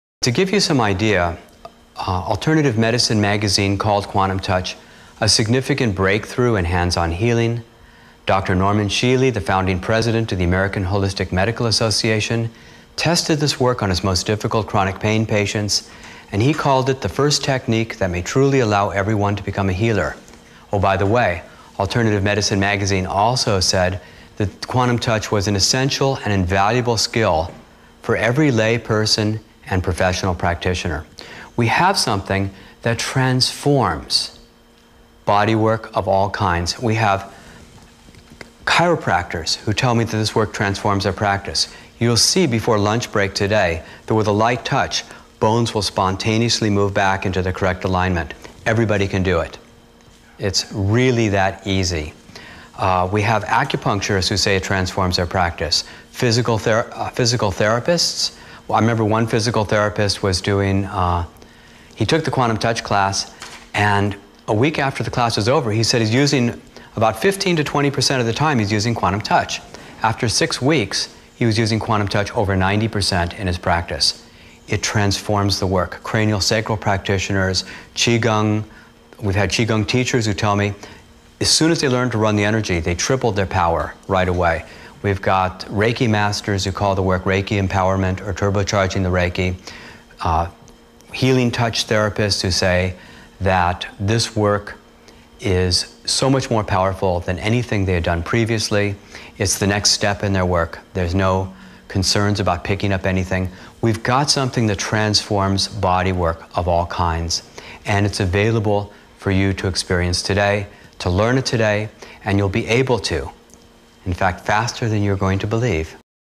😍 TOUCH HEALING!!!! how to do it - Quantum Touch
👇  CLICK SHOW MORE 👇
👇I really respect this science here and i think touch healing is quite an untapped resource i will soon be going to a lecture to check out how good this really is
obviously i think it really depends on the person ... so although they do tend to promise it's so easy and anyone can do it ... i still find that part very hard to accept - i think it takes years of training ... for most of us ... but worth a look :) please enjoy |